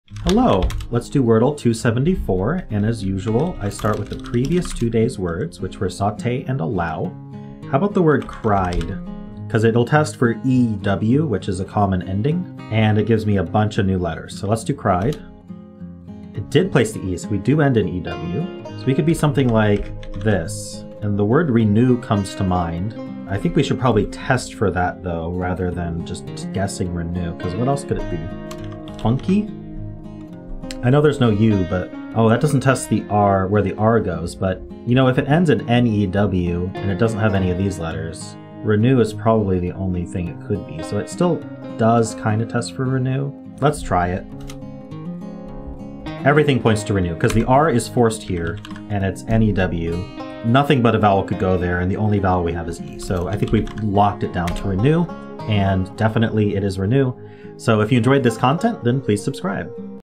Using guesses effectively!
(Spoilers 274) For fun, I start with the previous two day's words!

LIVE STREAMS
Join me every day for a word games and puzzles stream!